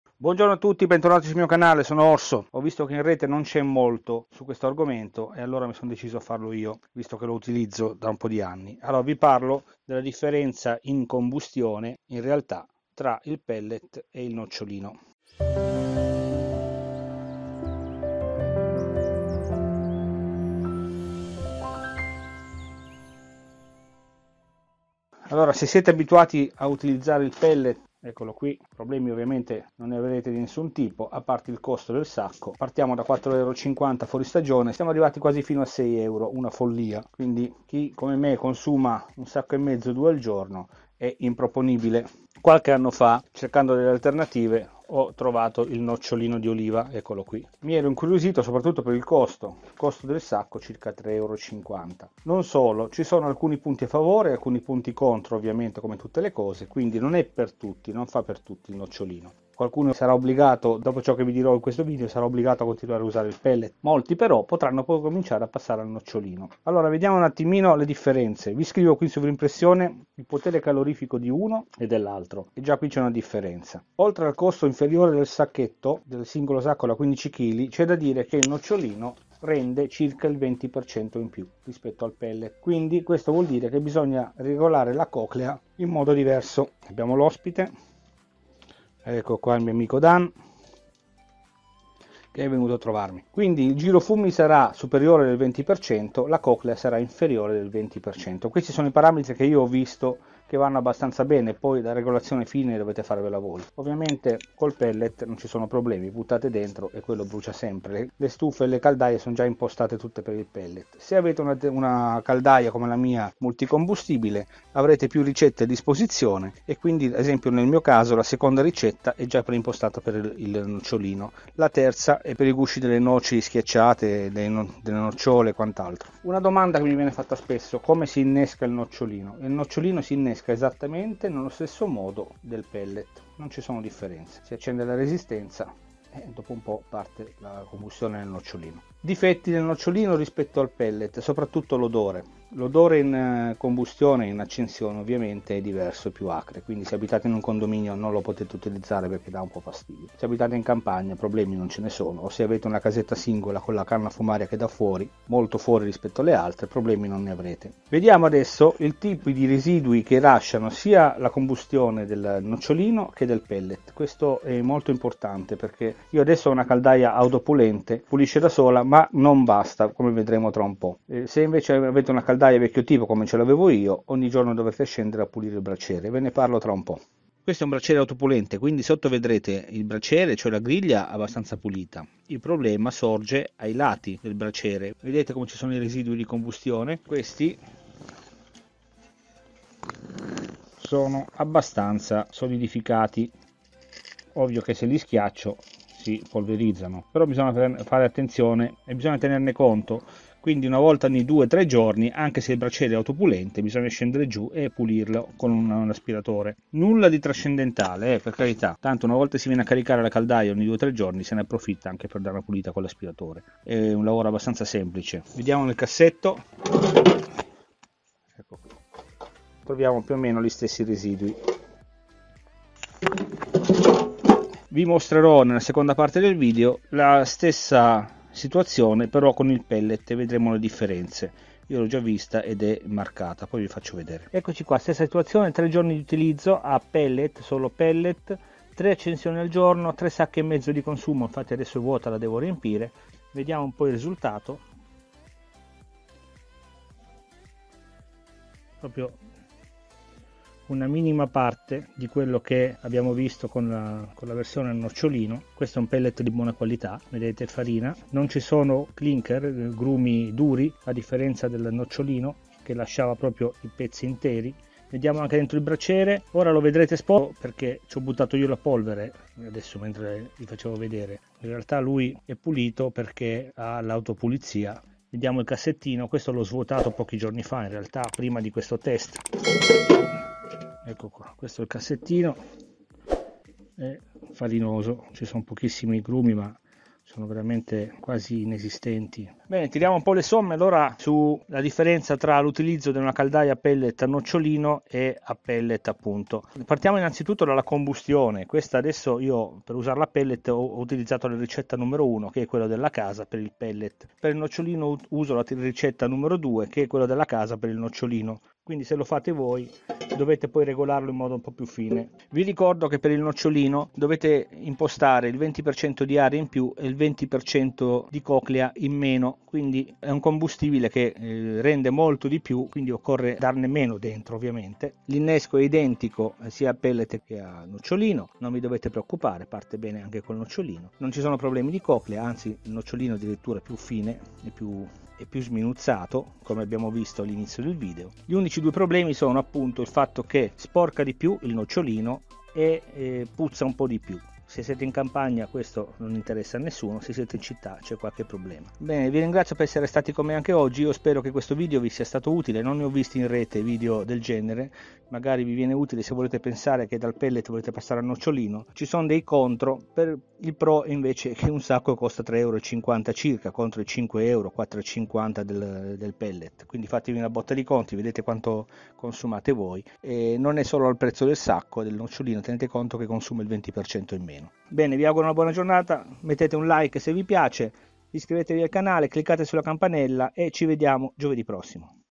Pellet contro Nocciolino: analisi dei residui di combustione
Se volete passare dal pellet al nocciolino vi consiglio di guardare prima questo video!